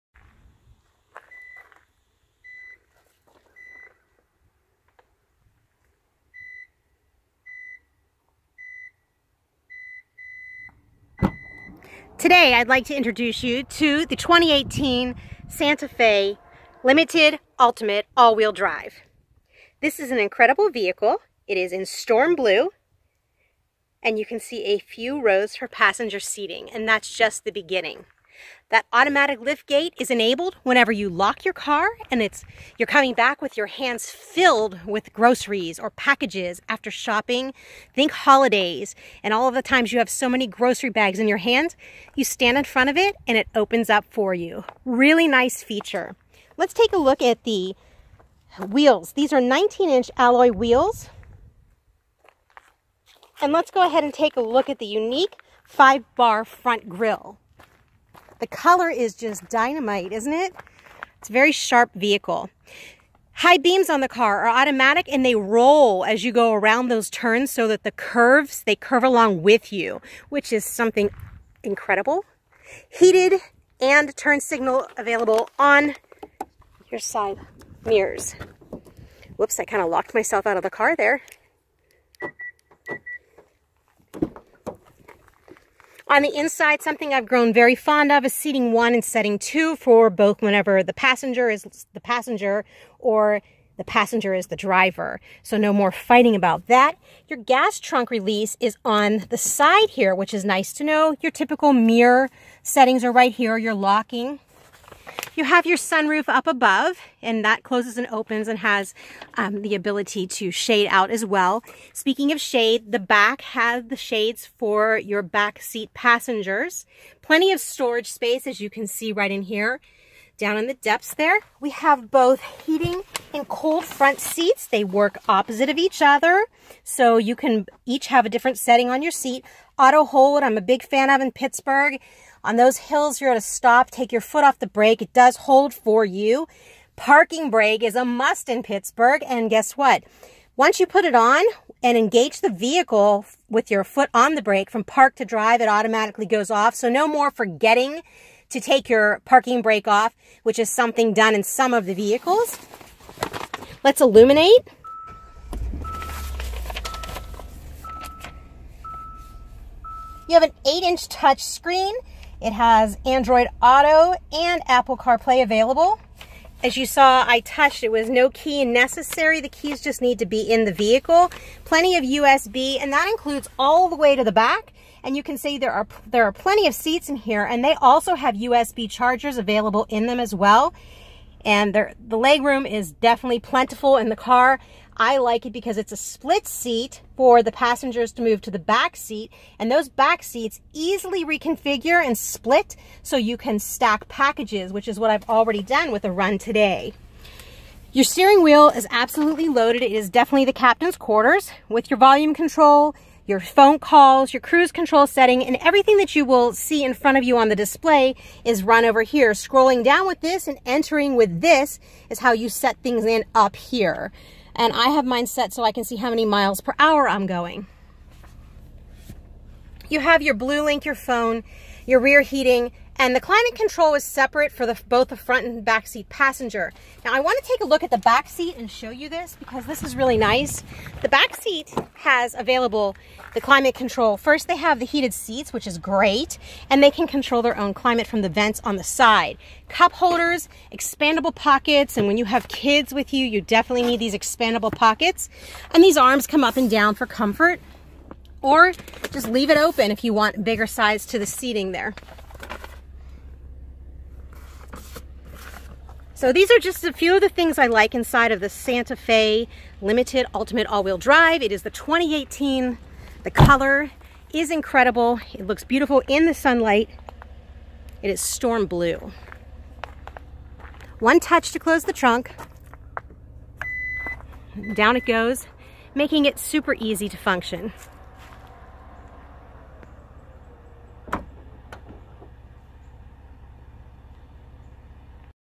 2018 Hyundai Santa Fe Ltd Ult AWD
An overview of the 2018 Hyundai Santa Fe Ltd Ult AWD - in Pittsburgh.